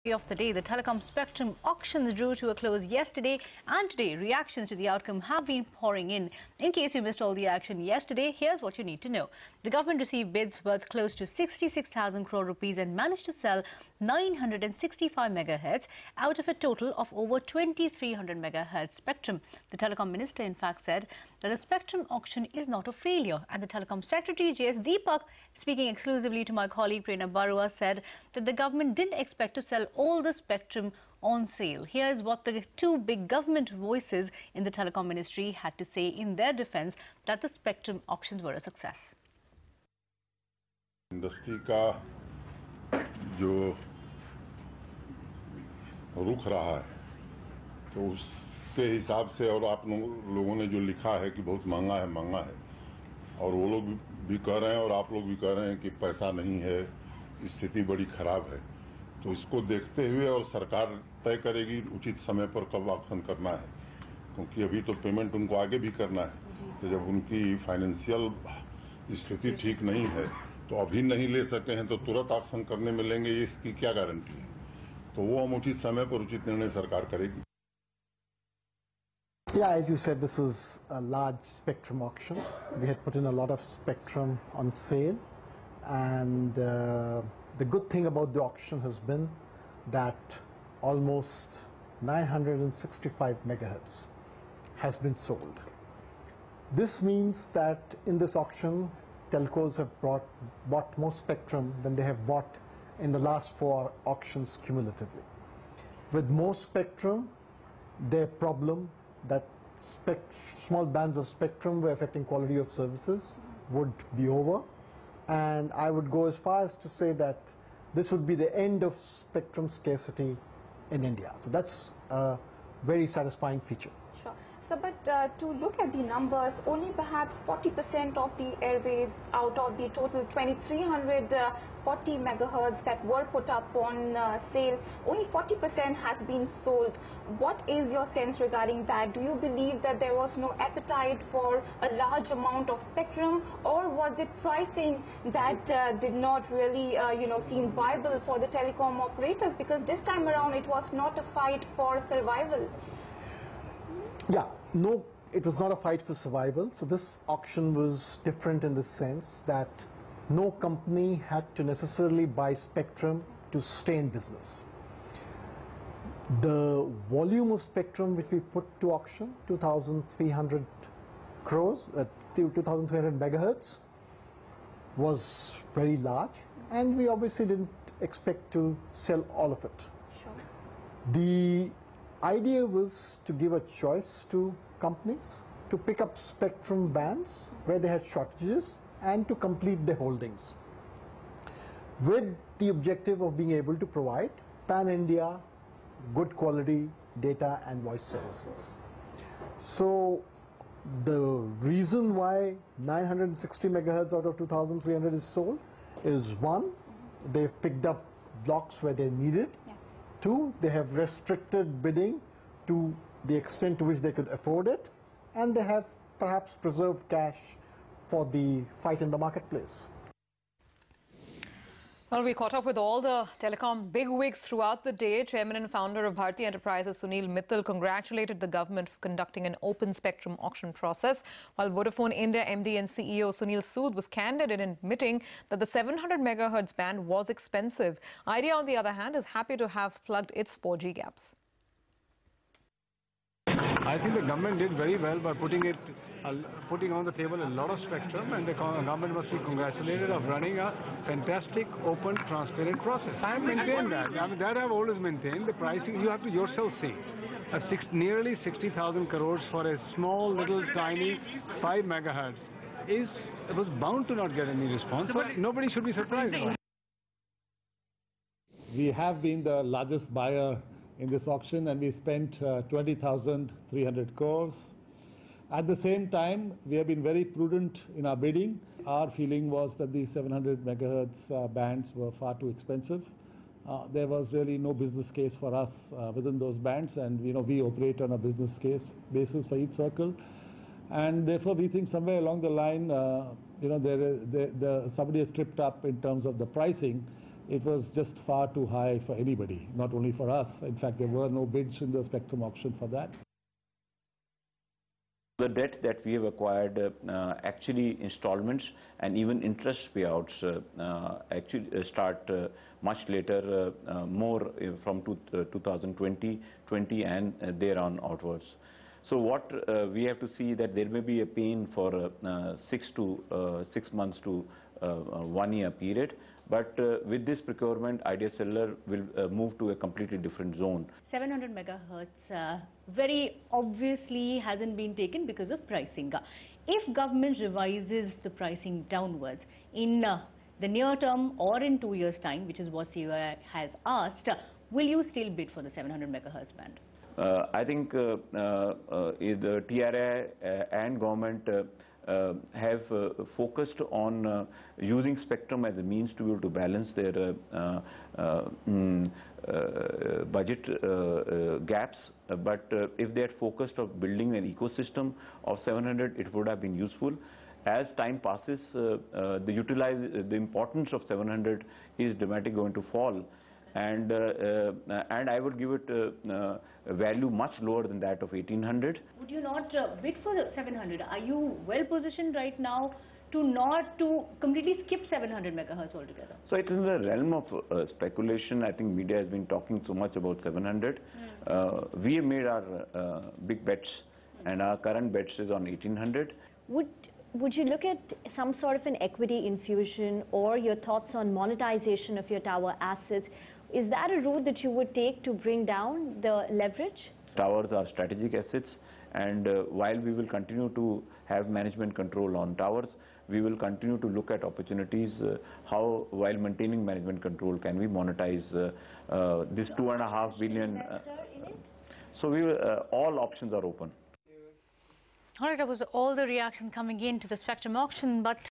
MOUNTING A DEFENCE. Highest Spectrum Sale In 5 Years: Telecom Min
The telecom spectrum auction drew to a close yesterday and today reactions to the outcome have been pouring in. In case you missed all the action yesterday, the government received bids worth close to 66,000 crore rupees and managed to sell 965 MegaHertz out of a total of over 2300 MHz. The Telecom Minister said that the spectrum auction is not a failure and the Telecom Secretary JS Deepak speaking exclusively to CNBC-TV18 said that the govt didn't expect to sell all the spectrum on sale.  Listen in.